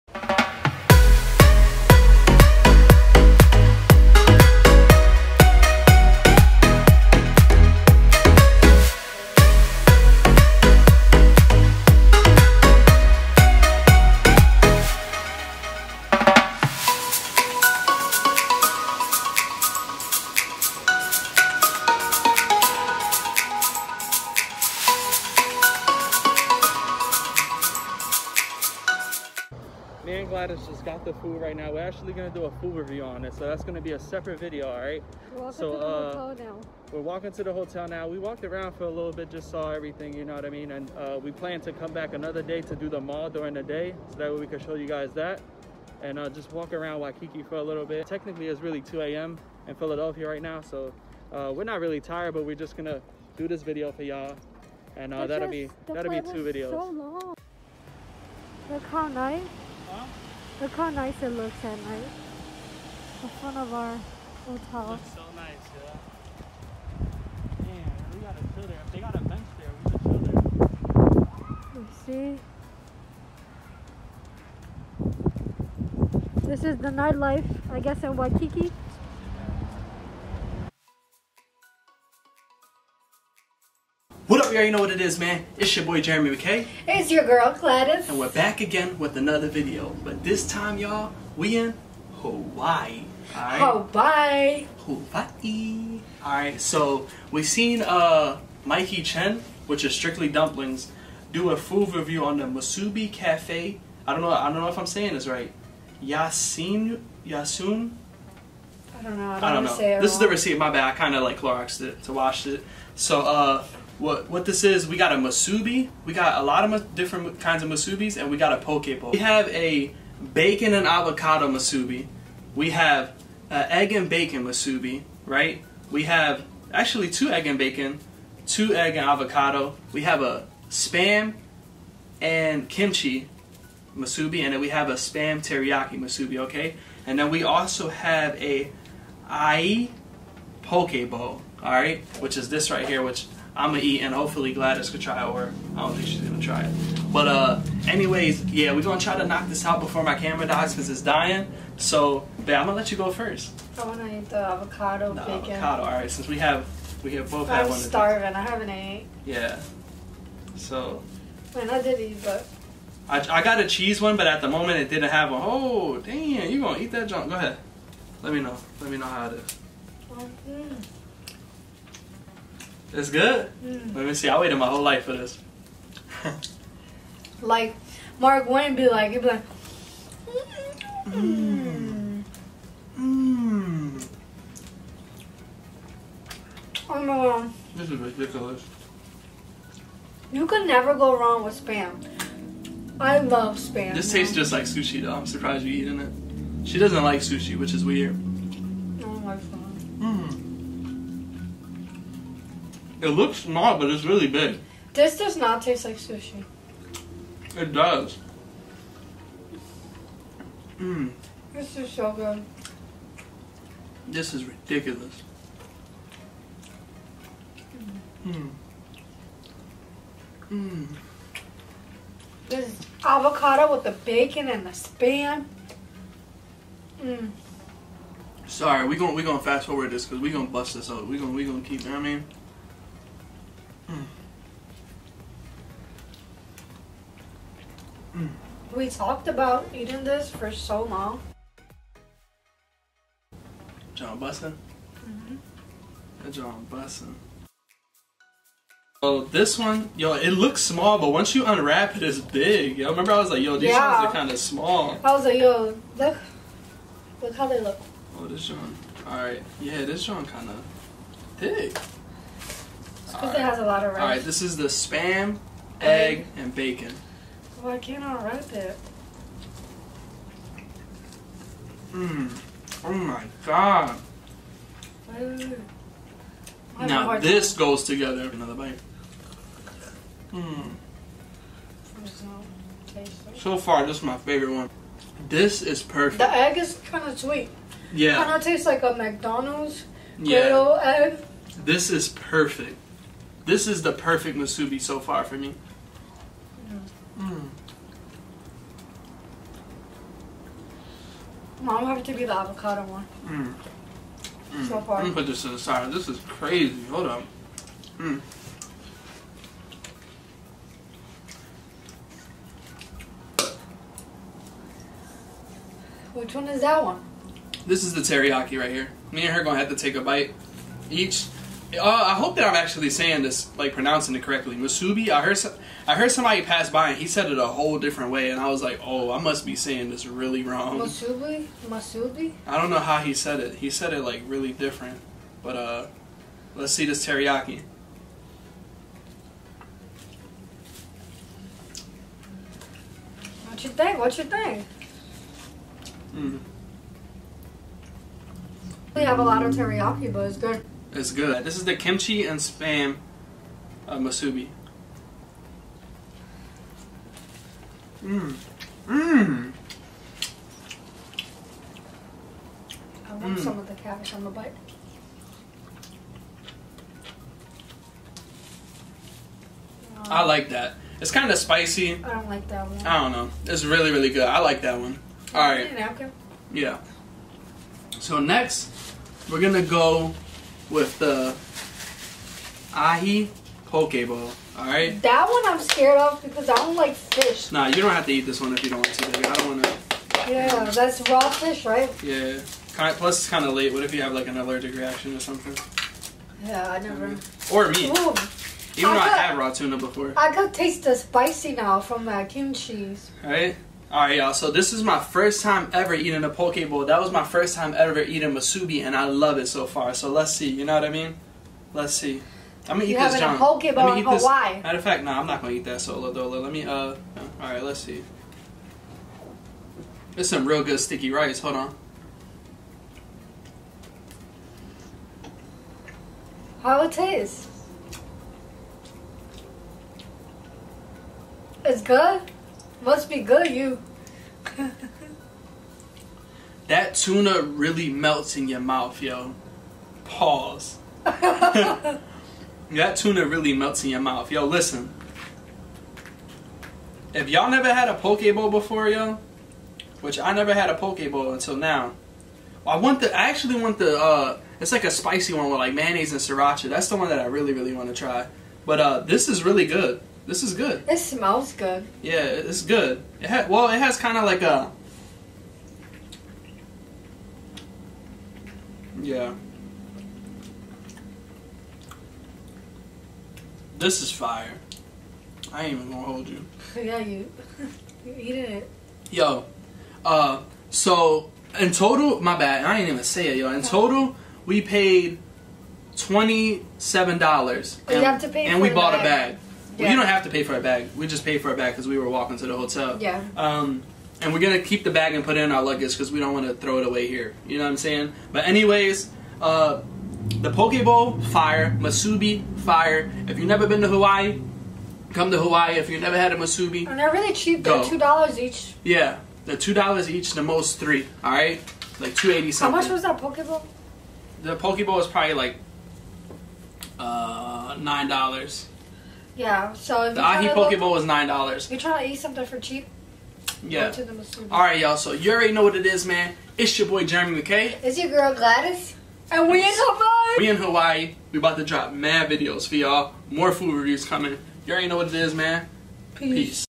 Local Hawaiian food | Musubi Cafe Iyasume Review | Hawaii Travel Vlog
I wanted a Spam musubi ... So I told Gladys to pack and lets go to Hawaii ! After arriving to Hawaii we decided to try the famous Musubi cafe in Waikiki Hawaii ! I loved it and I will miss it :( enjoy the video!! More Hawaii videos on the wayyyyyyy!

SC: Jeremymckay